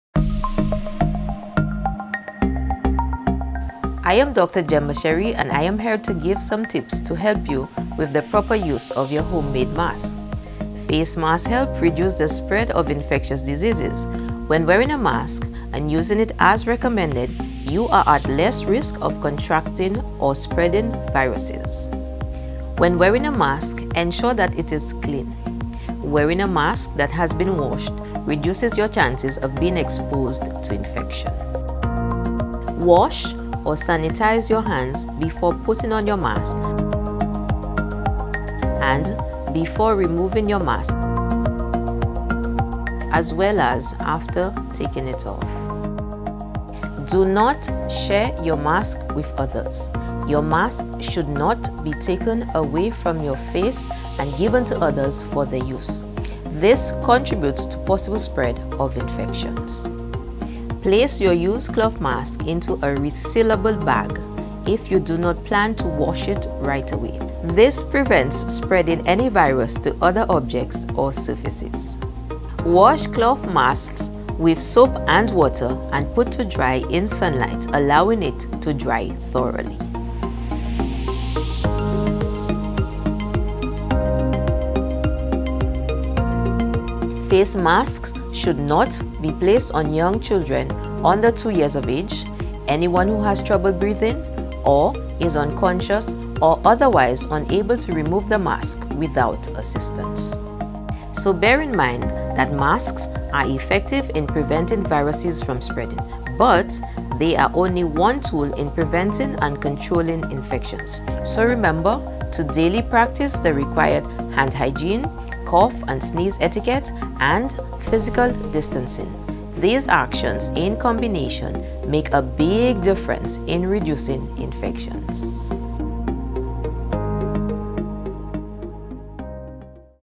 Proper use of face masks
Dr. Gemma Chery explains the proper use of home made masks.